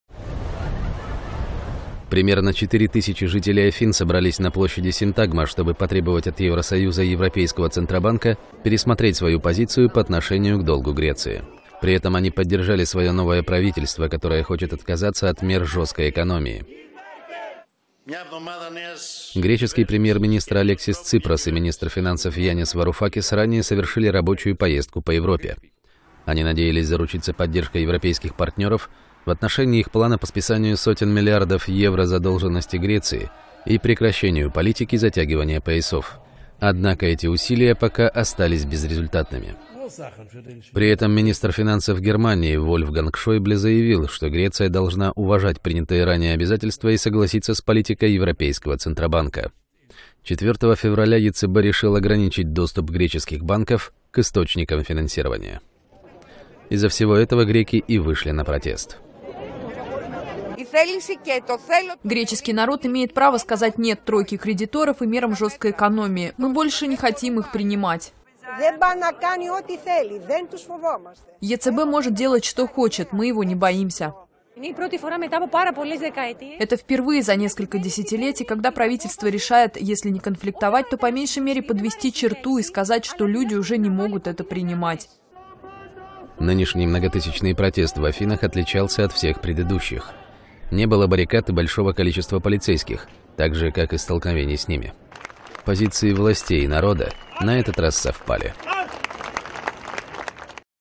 В Афинах требуют от ЕС пересмотреть долг Греции (новости)
В Афинах требуют от ЕС пересмотреть долг Греции. Примерно 4 тысячи жителей Афин собрались на площади Синтагма, чтобы потребовать от Евросоюза и Европейского Центробанка пересмотреть свою позицию по отношению к долгу Греции. При этом они поддержали своё новое правительство, которое хочет отказаться от мер жёсткой экономии.

Греческий премьер-министр Алексис Ципрас и министр финансов Янис Варуфакис ранее совершили рабочую поездку по Европе. Они надеялись заручиться поддержкой европейских партнёров в отношении их плана по списанию сотен миллиардов евро задолженности Греции и прекращению политики «затягивания поясов». Однако эти усилия пока остались безрезультатными.

При этом министр финансов Германии Вольфганг Шойбле заявил, что Греция должна уважать принятые ранее обязательства и согласиться с политикой Европейского Центробанка. 4 февраля ЕЦБ решил ограничить доступ греческих банков к источникам финансирования.

Из-за всего этого греки и вышли на протест.

[протестующая]:
«Греческий народ имеет право сказать «нет» «тройке» (ЕС, МВФ и ЕЦБ) и мерам жёсткой экономии. Мы больше не хотим их принимать».

[протестующая]:
«Он (ЕЦБ) может делать, что хочет. Мы его не боимся».

[протестующая]:
«Это впервые за несколько десятилетий, когда правительство решает, если не конфликтовать, то, по меньшей мере, подвести черту и сказать, что люди уже не могут это принимать».

Нынешний многотысячный протест в Афинах отличался от всех предыдущих. Не было баррикад и большого числа полицейских, также как и столкновений с ними. Позиции властей и народа на этот раз совпали.